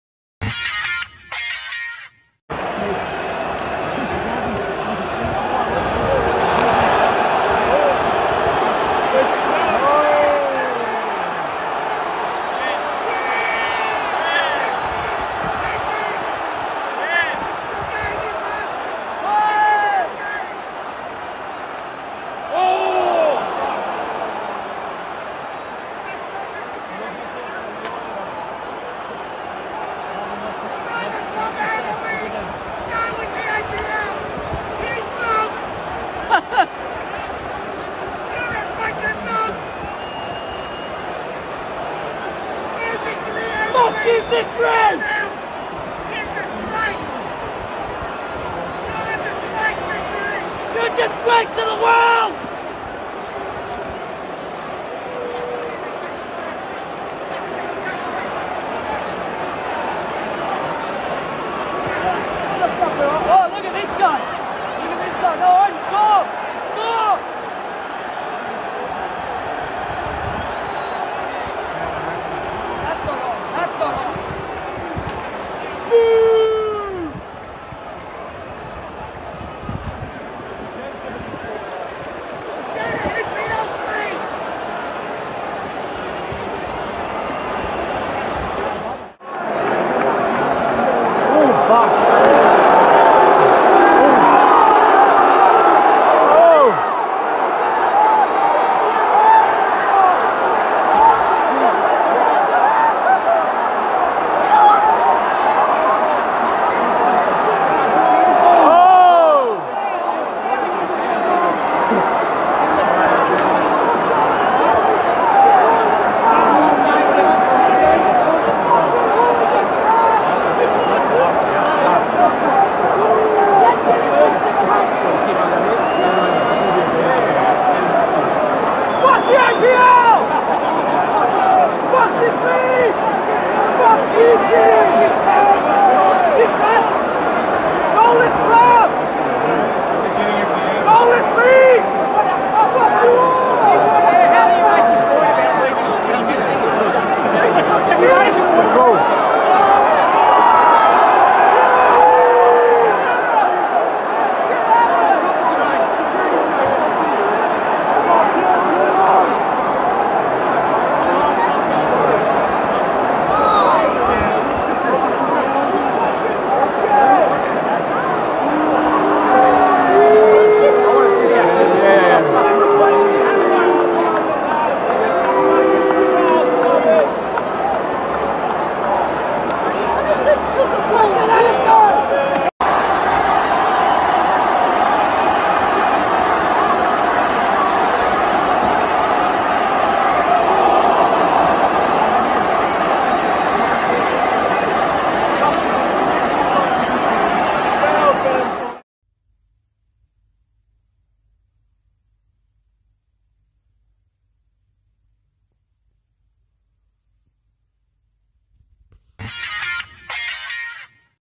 A-League pitch Invasion | Terrible Moments in Melbourne Derby in Australia
Terrible moments in Australian top-flight league in the derby clash between Melbourne Victory and Melbourne City.